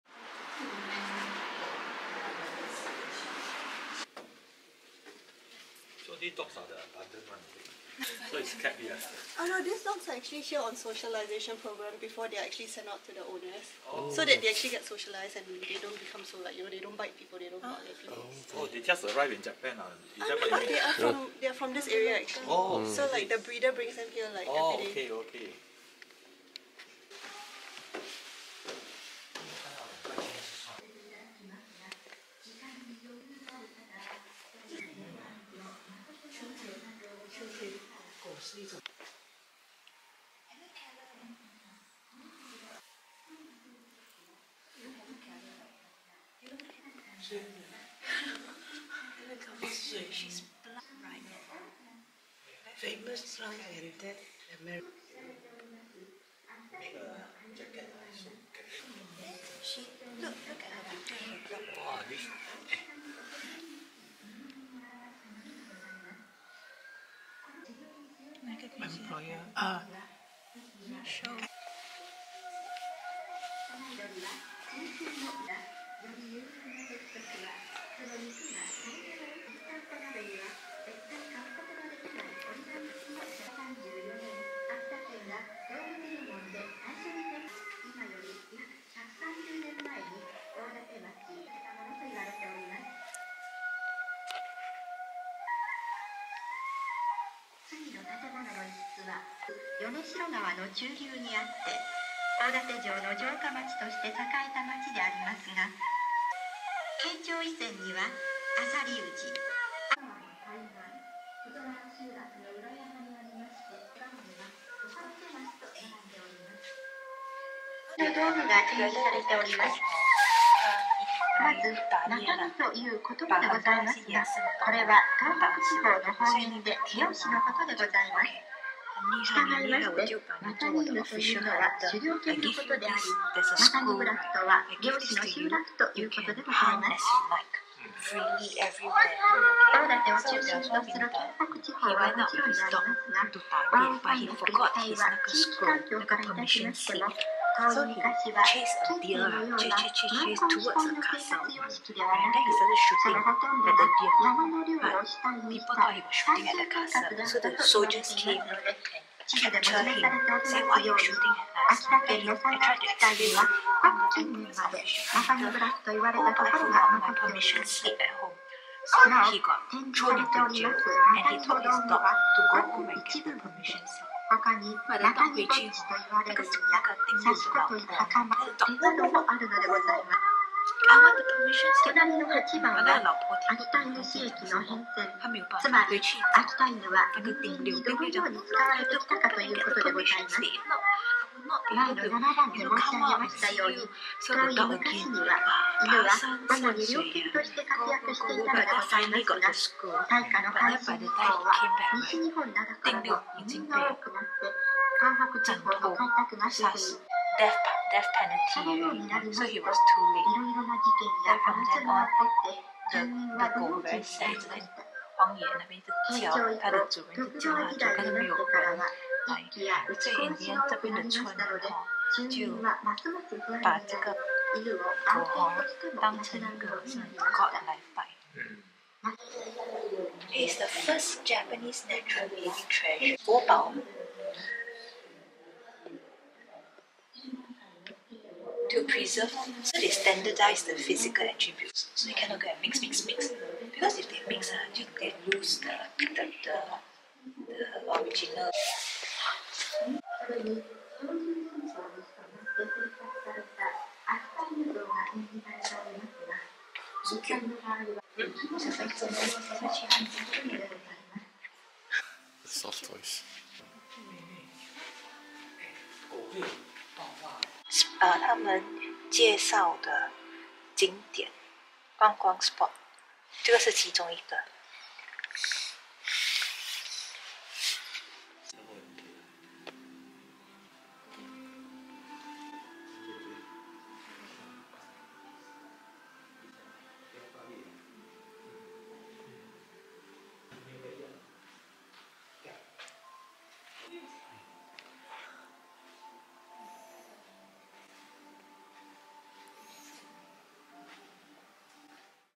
Journey into Canine History: Exploring Akita Dog Museum
Embark on a heartfelt exploration of the Akita Dog Museum with me, delving into the rich history and charm of Japan's beloved Akita dogs. Discover the unique exhibits, from the fascinating canine exhibits to heartwarming stories of loyalty and companionship. Join the call to support the preservation of Akita dogs, ensuring their legacy for future generations. This is not just a museum visit; it's an opportunity to be part of the mission to celebrate, cherish, and preserve the incredible heritage of these remarkable canine companions.